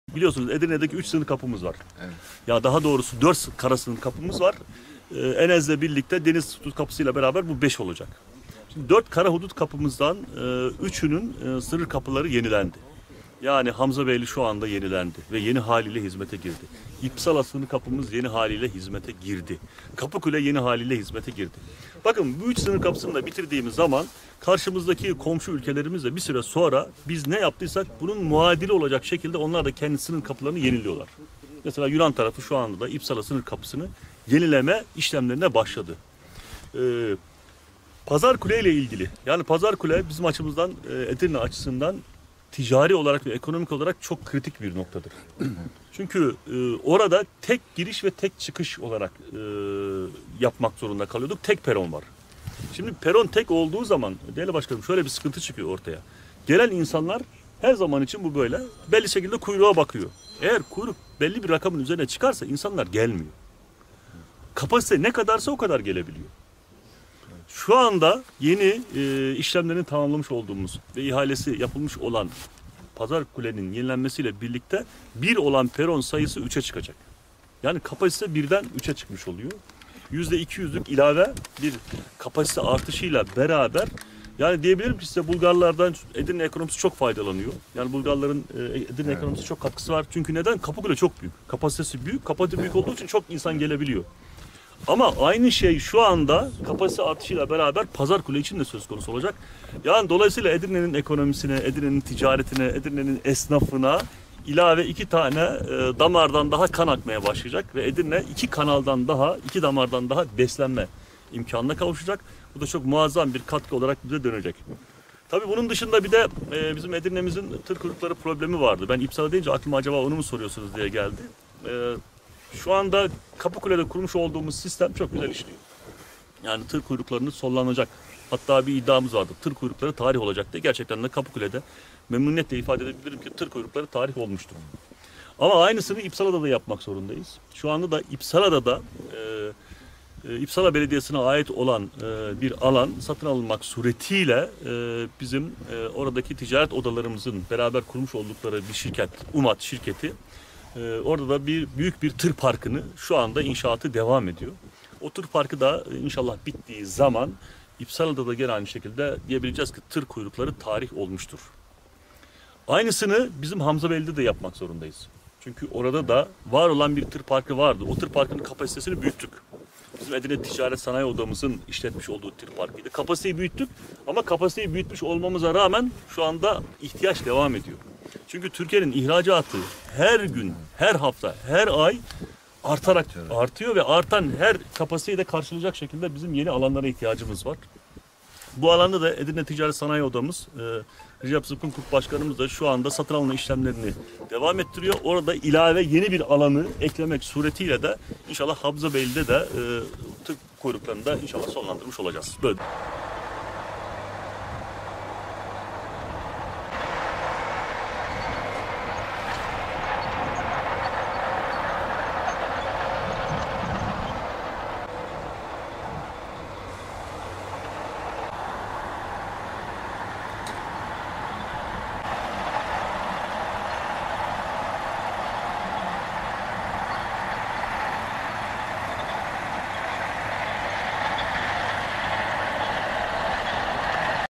Kur yükseldi, Edirne'ye alışverişe gelenlerin sayısı arttı
Euro’nun 12.5 TL, Bulgar Levası’nın ise 6,5 TL’yi görmesi ile birlikte Yunanistan ve Bulgaristan’dan Edirne’ye alışverişe gelenlerin sayısı arttı Pazarkule sınır kapısındaki yoğunluğa işaret eden Edirne Valisi Ekrem Canalp, sınır kapısının kapasitesinin yüzde 200 oranında artırılacağını açıkladı.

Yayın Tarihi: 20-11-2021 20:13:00